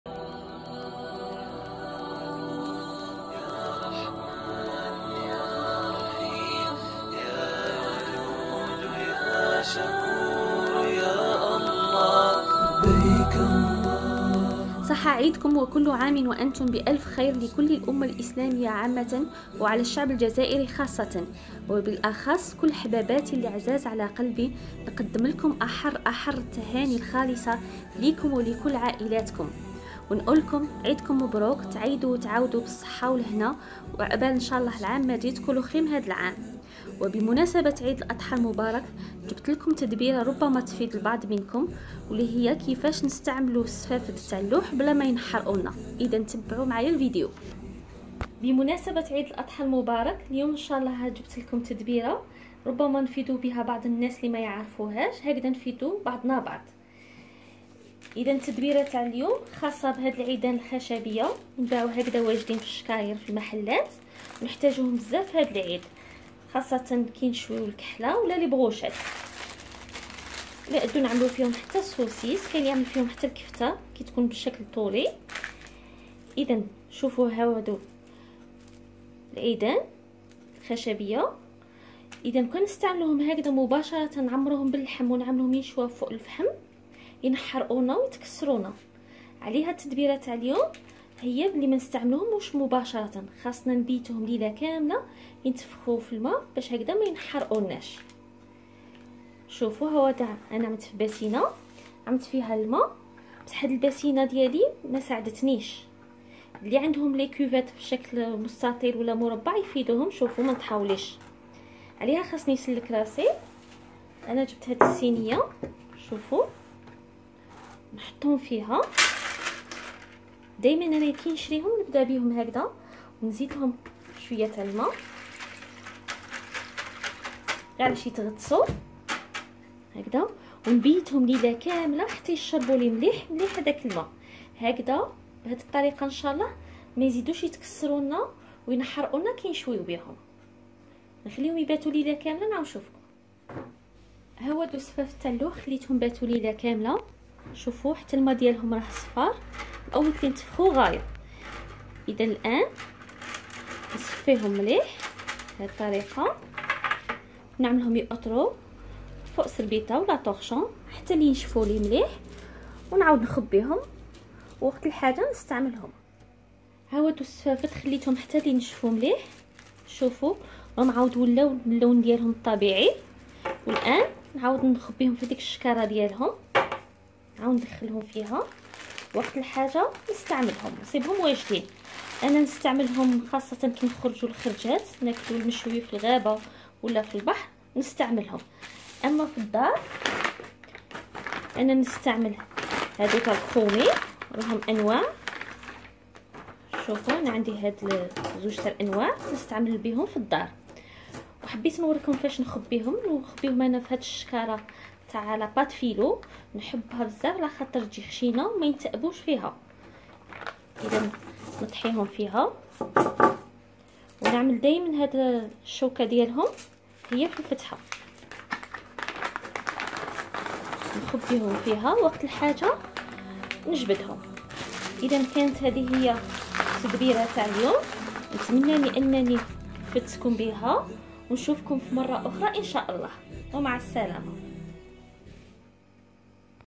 مطبخ تلمسان : إستعمال الصحيح ل عود الشواء Méthode Juste pour Utiliser Oud Ch'wa
الف الف مرحبا بكل من يتابع فيديو اليوم و فيديو هو عبارة عن تدبيرة خاصة بالعيدان الخشبية الخاصة بالشواء بطريقة صحيحة باش ما ينحرقوش اثاء وضعهم على الفحم .